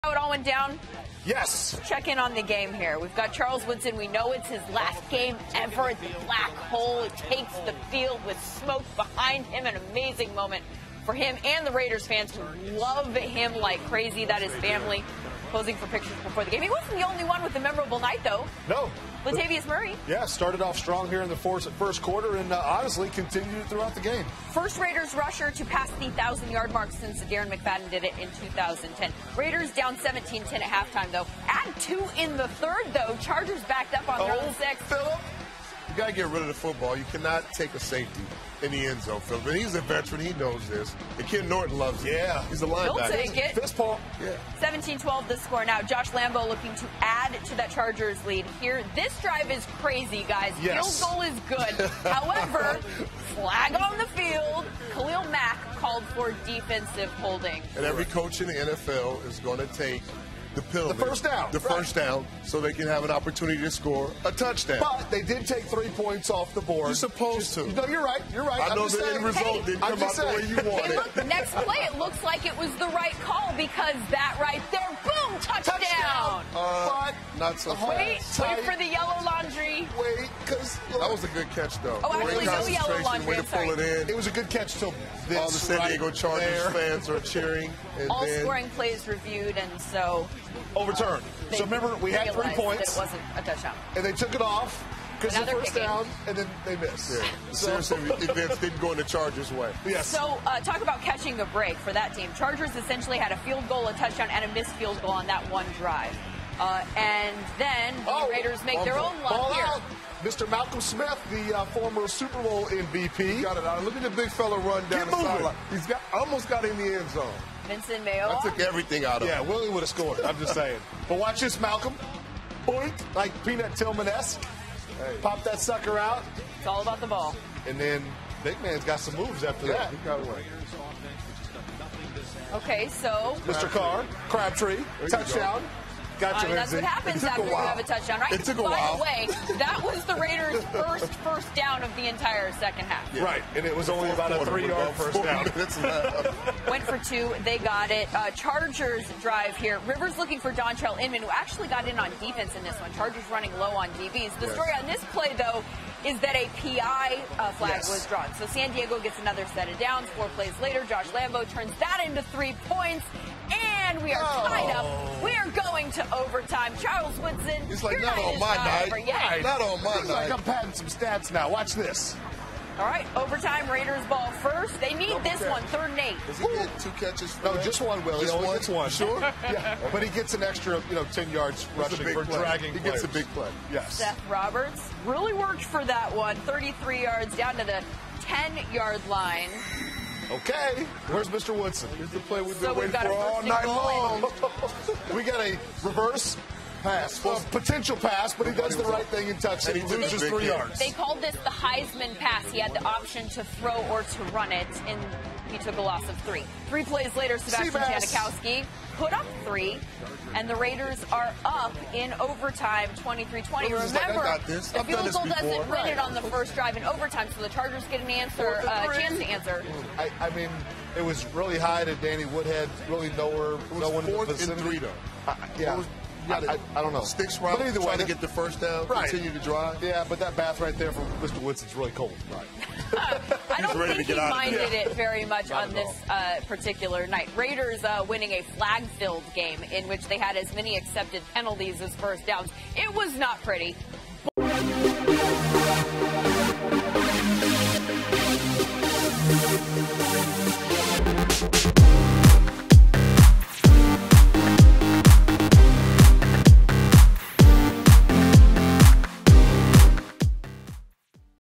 Chargers vs. Raiders | Week 16 Highlights | NFL
Check out Week 16 highlights between the San Diego Chargers and the Oakland Raiders.